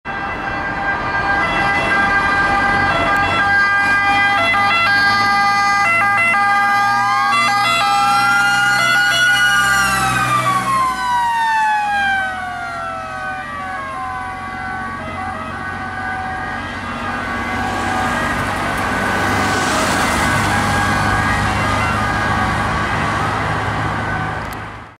[SIRENA FISCHIO] Autoscala Magirus-Deutz Vigili del Fuoco di Mestre in emergenza - VERY RARE
La seconda AutoScala (utilizzata anche come riserva) su telaio Iveco Magirus-Deutz 192D13 dei Vigili del Fuoco di Mestre si dirige verso il target d'intervento con i dispositivi luminosi e sonori attivi.
L'AutoScala è dotata di doppi dispositivi sonori, sirena bitonale tono soccorso e come dispositivo secondario una sirena a fischio.
Si tratta di un mezzo acquistato in Alto Adige dal Corpo Volontario di Silandro-Schlanders (BZ).

Modello: Iveco Magirus-Deutz 192D13
Allestimento: Magirus

Telecamera: Sony HDR-CX 240

*ENGLISH*
Italian fire brigade's ladder Iveco Magirus-Deutz 192D13 responding to a call with lights and siren.